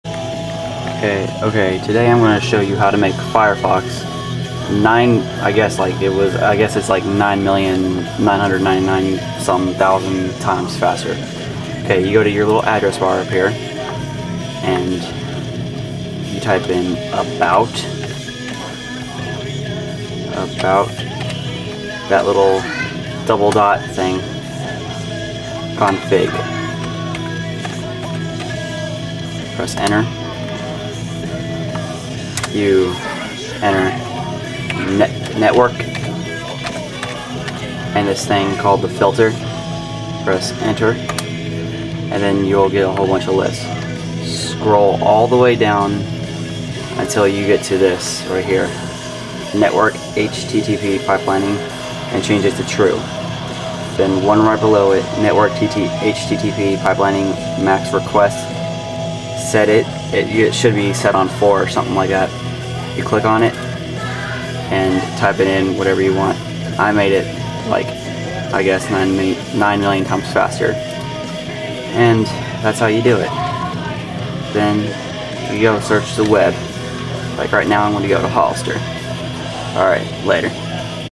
How To Make FireFox Go 9999999 Times Faster!!!
this is how you make firefox web browser go 9999999 times faster.